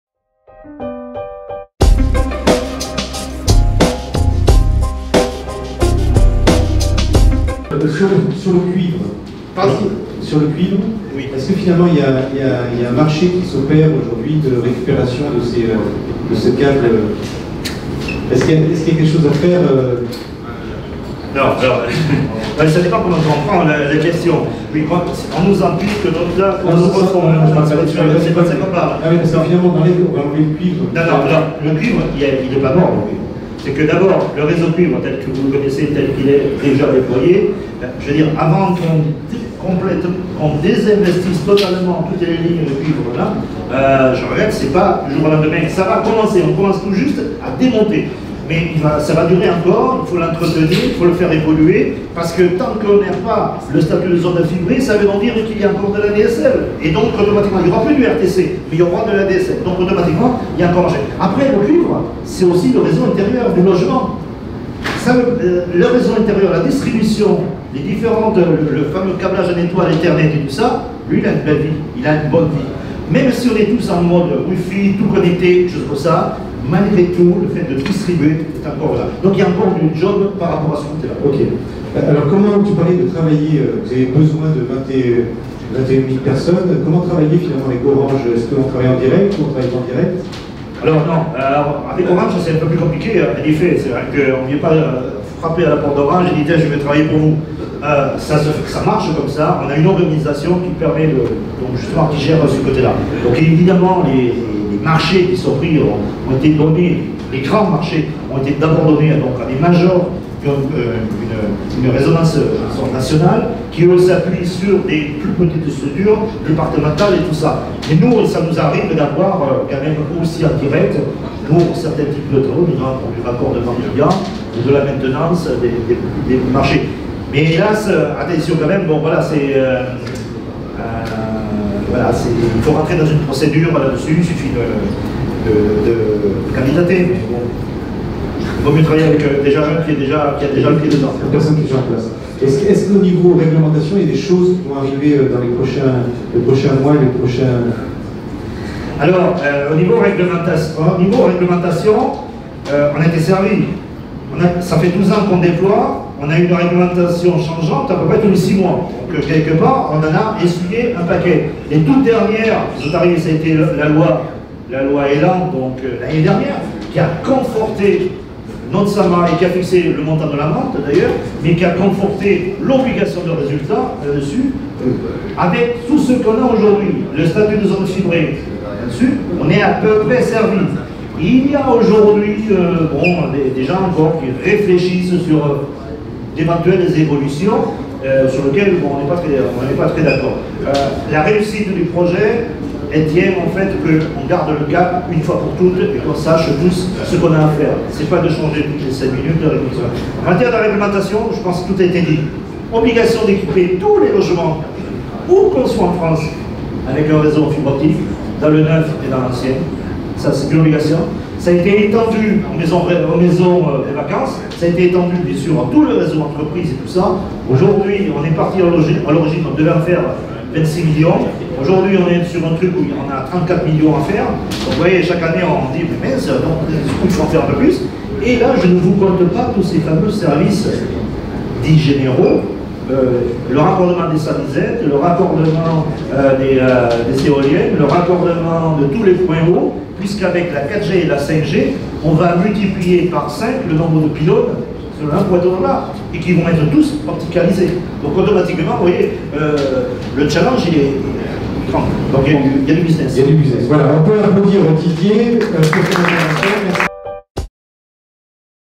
Fibre optique : quel avenir pour les électriciens ? (questions d'experts avec Orange)
Questions d'experts à Didier Cazes, responsable des relations externes de la Direction Fibre chez Orange, à l'occasion du Grand Circuit Énergiser le Bâtiment du 27 juin à Nice.